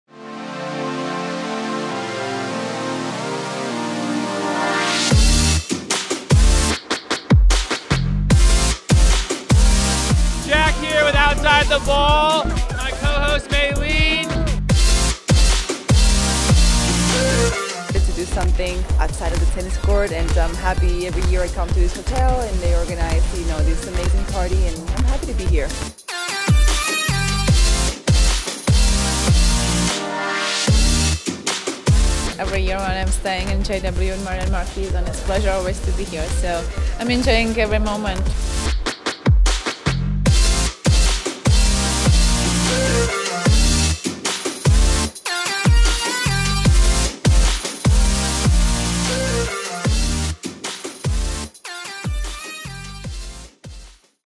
Win a Chance to Party With Tennis Stars in Miami
Last year the Match Point Passion Party was a buzz with tennis stars such as Garbine Muguruza, Petra Kvitova, Victoria Azarenka, Kei Nishikori and Jack Sock. Now in 2019, Outside the Ball is giving away an invitation for you + a friend to attend the exclusive party in Miami.

If you're in Miami on March 19th, this is a perfect opportunity to mingle with tennis players and dance the night away at the JW Marriott Marquis Miami hotel. To enter the giveaway, visit Outside the Ball on their social media platforms and follow the directions under the post on Facebook, Twitter, and Instagram.

The winner will be announced on March 15th.  Good luck, tennis fans. We look forward to you being our guest!

New videos every TennisTuesday!